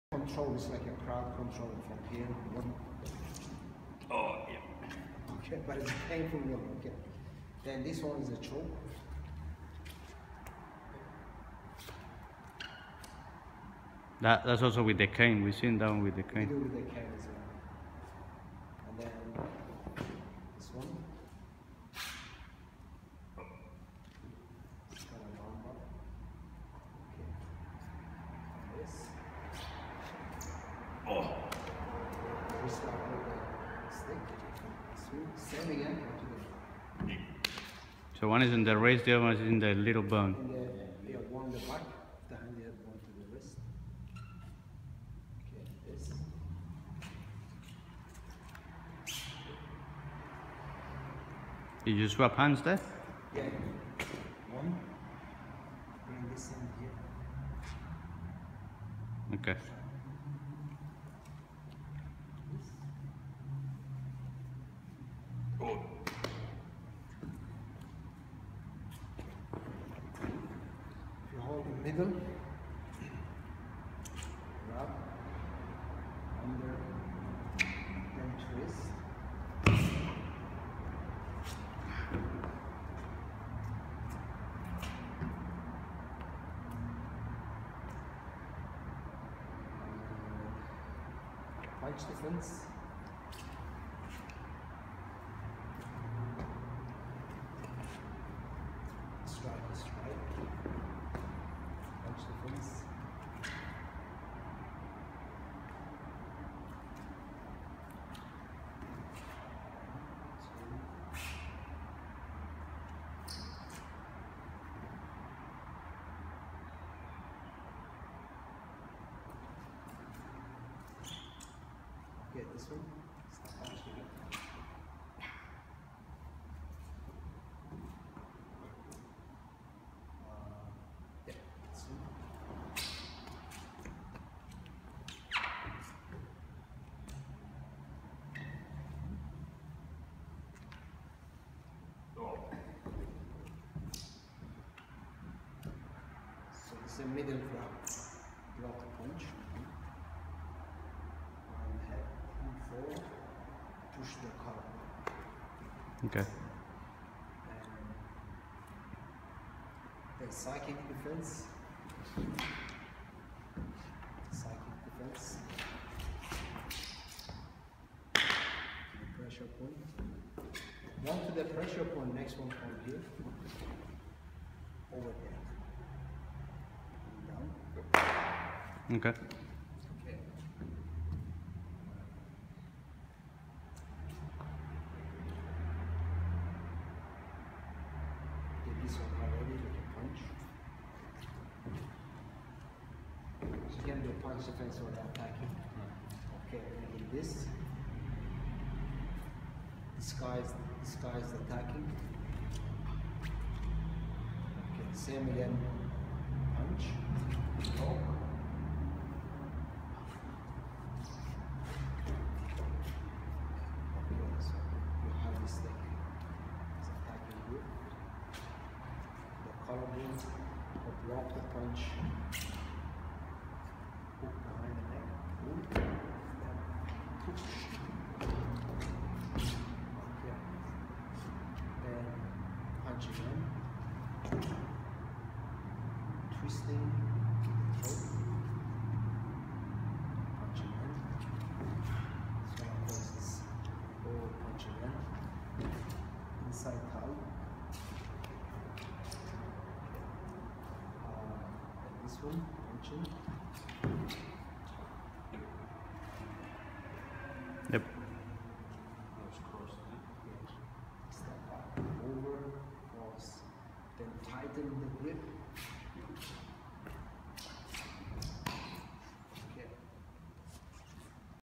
SinMoo Hapkido - Short Stick techniques - 2nd Dan Black Belt
Video of Grand Master Ghorbani demonstrating SinMoo Hapkido short stick techniques from 2nd Dan Black belt curriculum